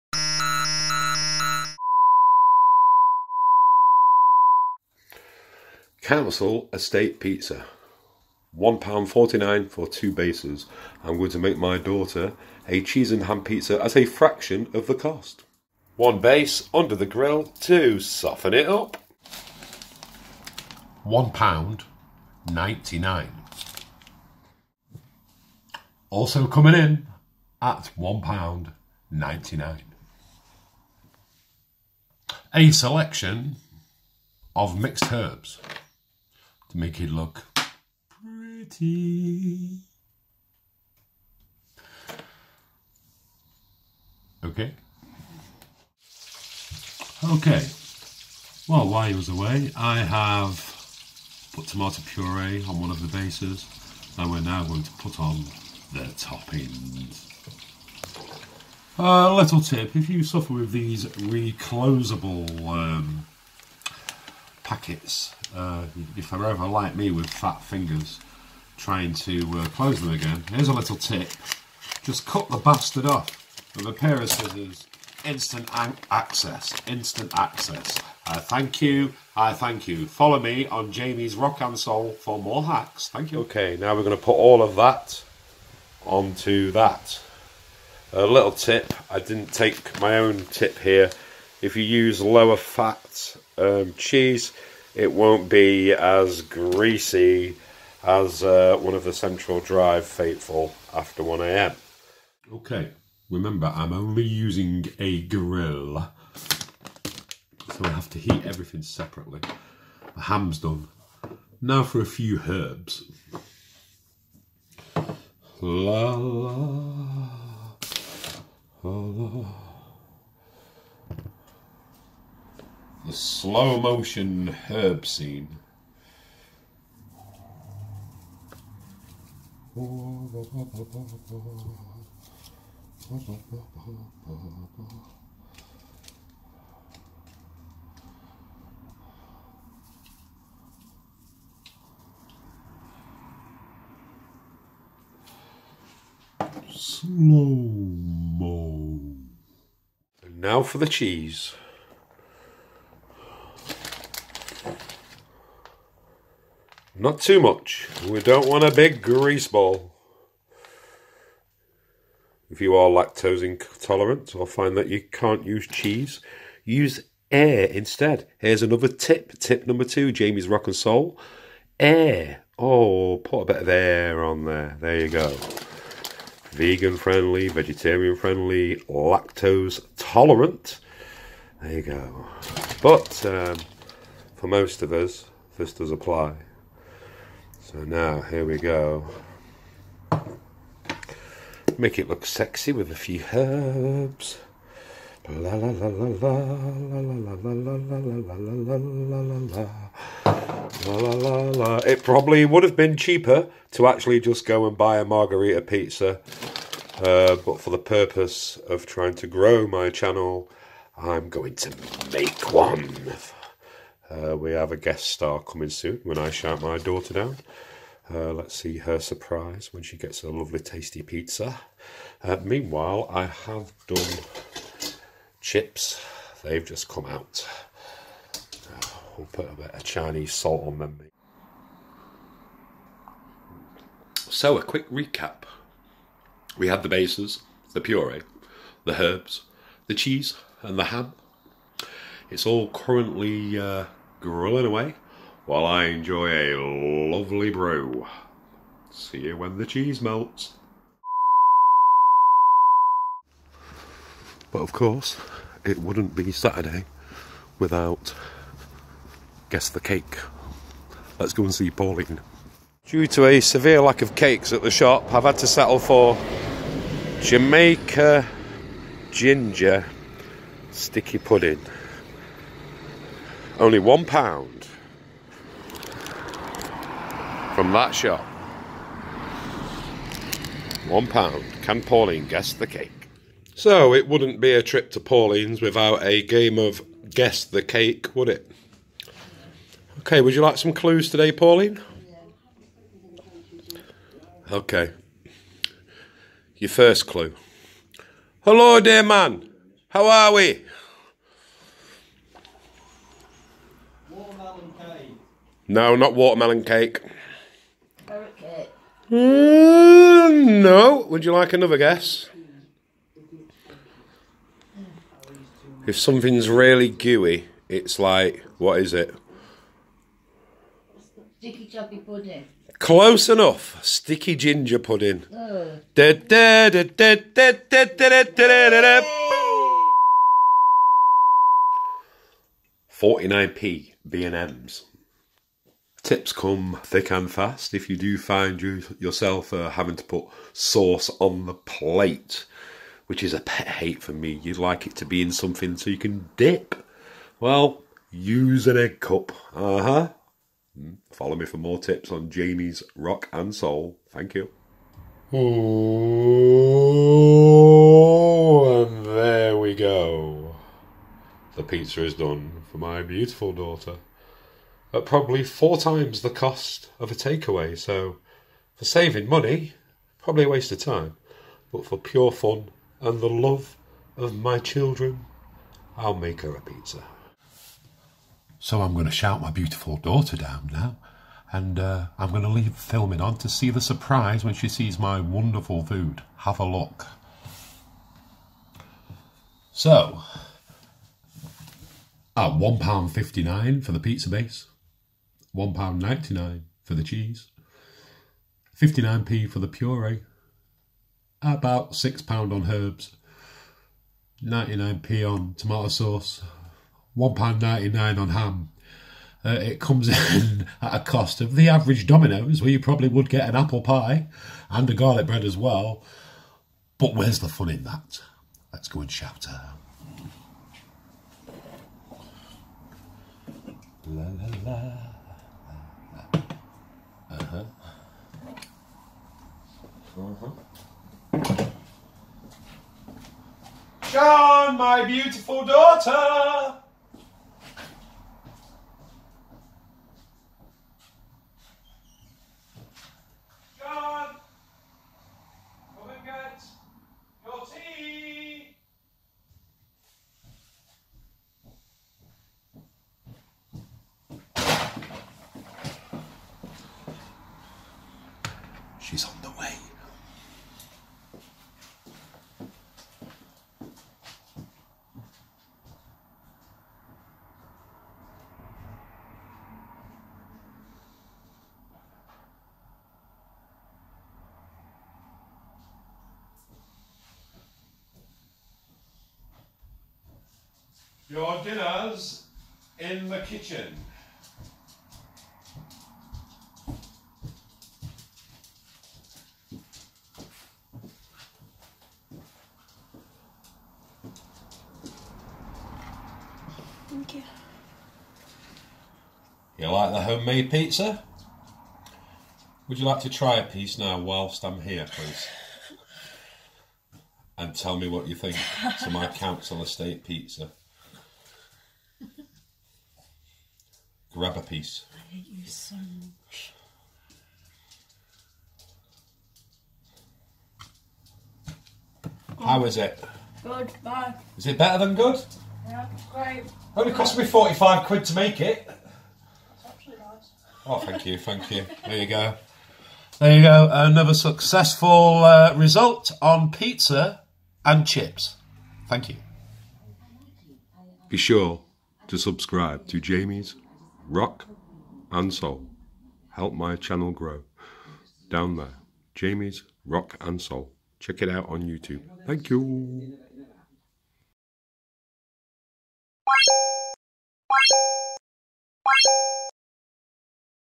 Home Made PEPPERONI Pizza Always Cheap & Tasty!!
Pizza always nice when you prepare them yourself, taste the savings!!! Council Cuisine style of course !!!